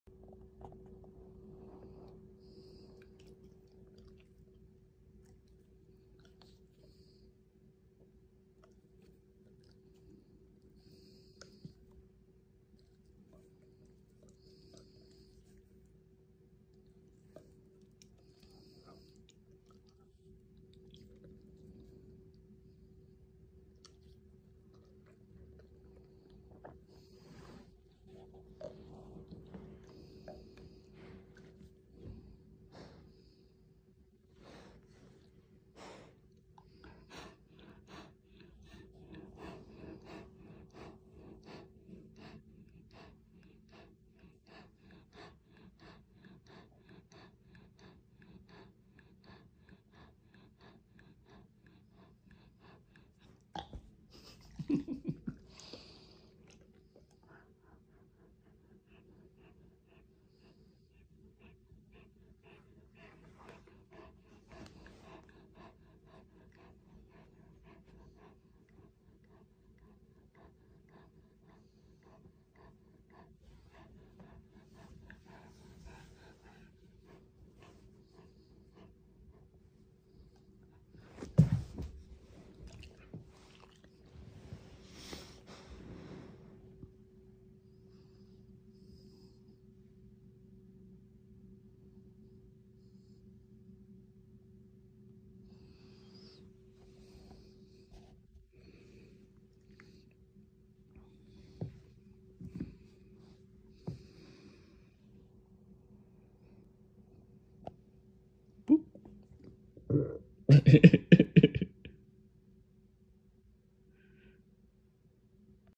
wolfhound close-up (sound on -- burps at 01:48) [2021-12-28]
Post-boop burp 01:48